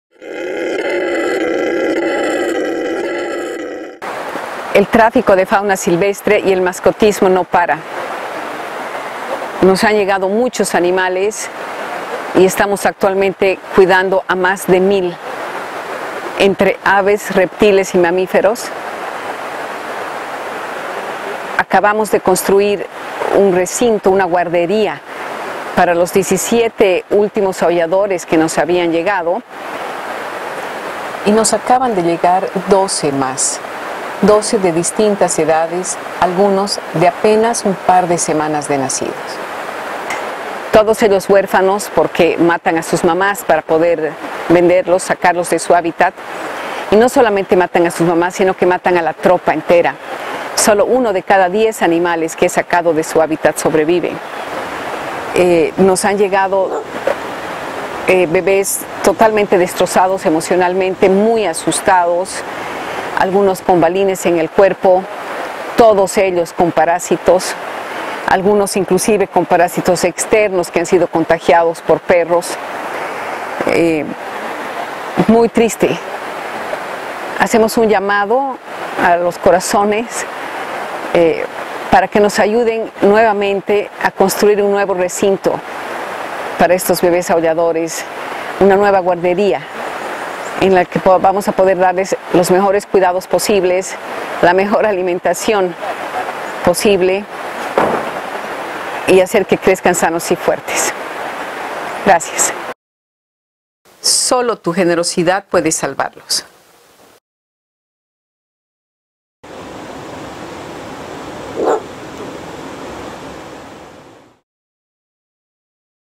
Baby Howler Monkeys Campaign
12 baby howler monkeys need your help!

They were just rescued from illegal trafficking and destruction of habitats, their mothers were killed, they are terrified, emotionally destroyed, sick and sad.

These babies need YOUR help and are in need of a healthy and safe nursery where they can survive and thrive. We need to raise $us 8,700 to build and furnish their new home. There is no better time than now to help these howlers who are survivors of the illegal wildlife trade and cruelty. Your gift will ensure they  are  kept warm, comfortable and healthy. It will also help us to prepare for the rescues to come ensuring we can keep running our rehabilitation programs on behave of those in desperate need.

Your gift goes a long way to ensure that we can continue to offer the support we need to rescued animals and help us fulfill our mission to give them a life true worth living.

The global wildlife crisis allows opportunity to us humans to rethink our action to future benefit the planet and restart for a new beginning. Let this give us hope!

Thank you for sharing hope for a more peaceful and finer  existence for all who share the Planet!

Vicky Ossio

Co Founder

Senda Verde